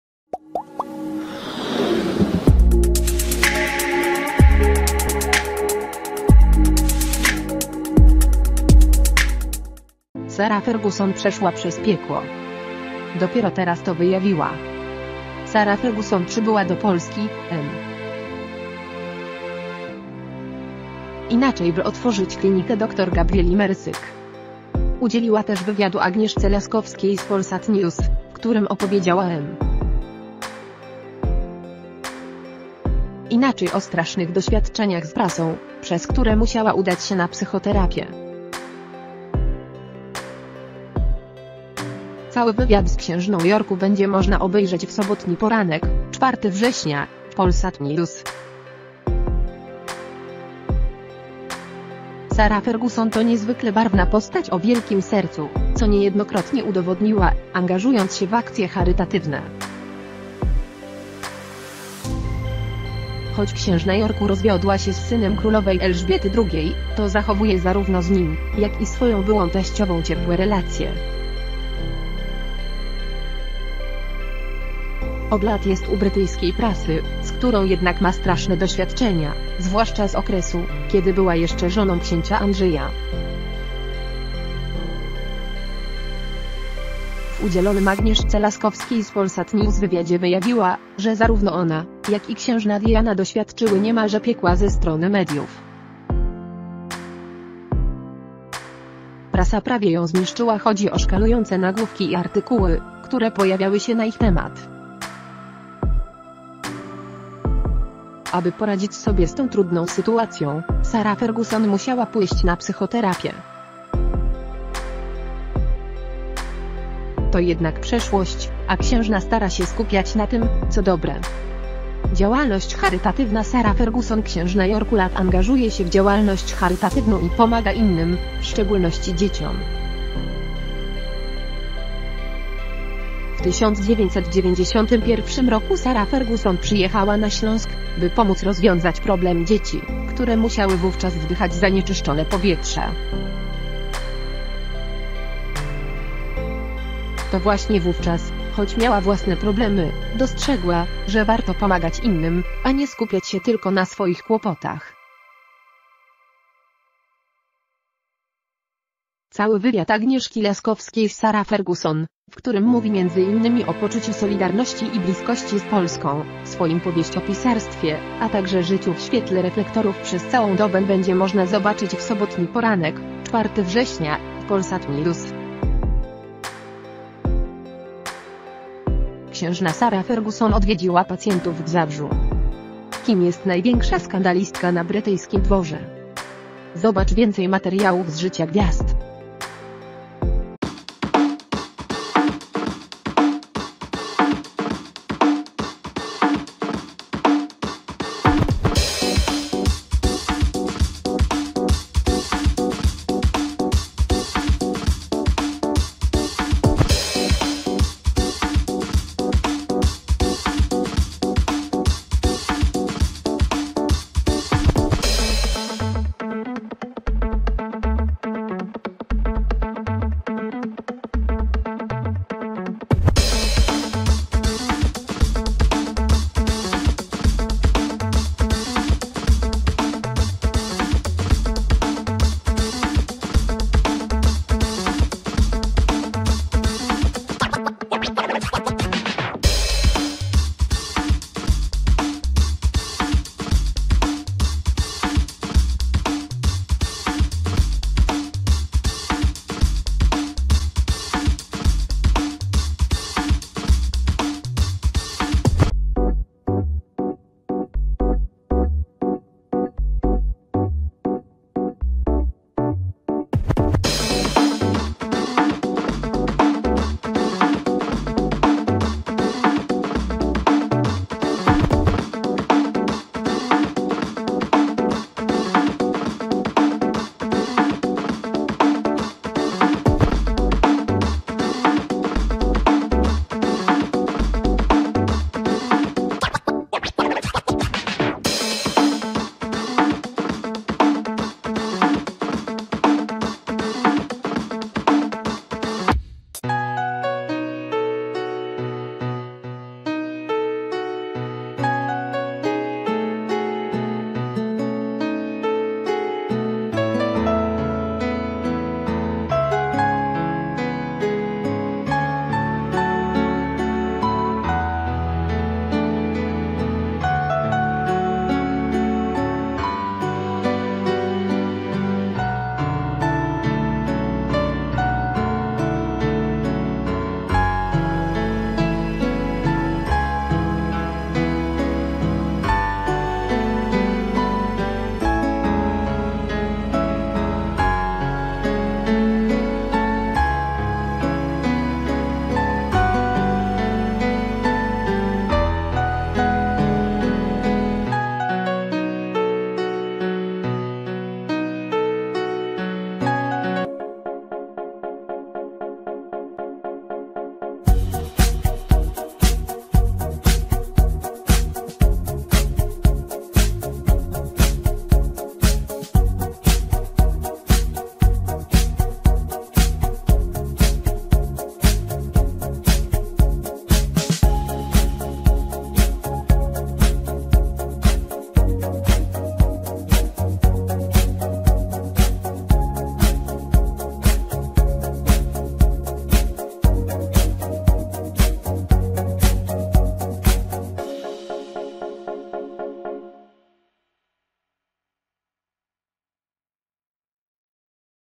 Sarah Ferguson przeszła przez piekło! Dopiero teraz to wyjawiła!
Sarah Ferguson przeszła przez piekło! Dopiero teraz to wyjawiła!  Sarah Ferguson przybyła do Polski, m.in. by otworzyć klinikę dr Gabrieli Mercik. Udzieliła też wywiadu Agnieszce Laskowskiej z Polsat News, w którym opowiedziała m.in. o strasznych doświadczeniach z prasą, przez które musiała udać się na psychoterapię. Cały wywiad z księżną Yorku będzie można obejrzeć w sobotni poranek, 4 września, w Polsat News.           Sarah Ferguson to niezwykle barwna postać o wielkim sercu, co niejednokrotn  ...